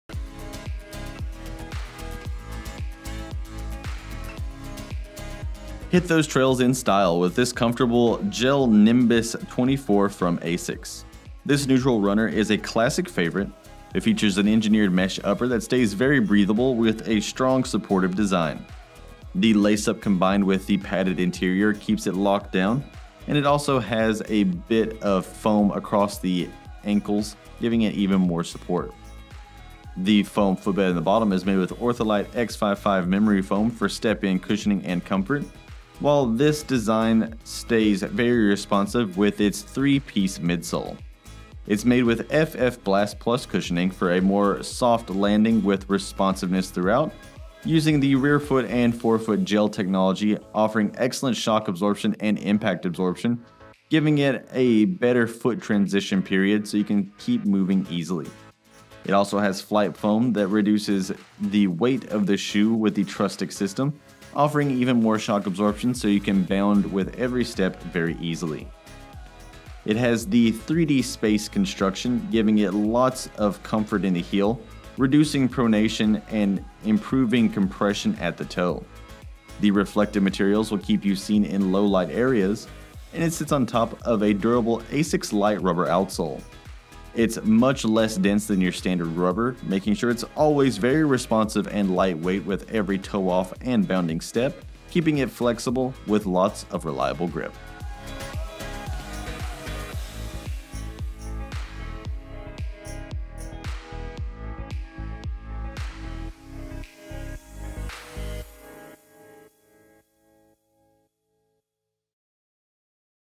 ASICS GEL-Nimbus® 24 Trail SKU: 9740030
Go the distance during your next trail run in the ASICS® GEL-Nimbus® 24 Trail sneakers.
Traditional lace-up closure offers a secure fit.
FF BLAST™ (FLYTEFOAM BLAST) is a lightweight, midsole foam with energetic and highly responsive cushioning and bounce.
OrthoLite™ X-55 sockliner improves comfort.
3D SPACE CONSTRUCTION™ Improves compression and cushioning at footstrike and reduces pronation.
Rearfoot and Forefoot GEL® Technology Cushioning System Attenuates shock during impact and toe-off phases, and allows movement in multiple planes as the foot transitions through the gait cycle.
Synthetic outsole.
Imported.